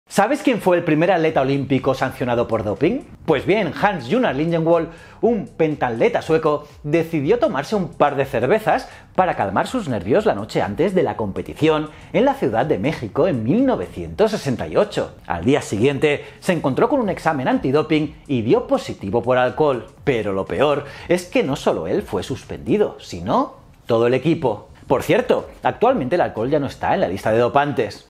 El PRIMER ATLETA SANCIONADO por DOPING
Hans-Gunnar Liljenwall (9 de julio de 1941) es un pentatleta sueco, quien causó la descalificación del equipo sueco en los Juegos Olímpicos de México 1968 por uso de alcohol.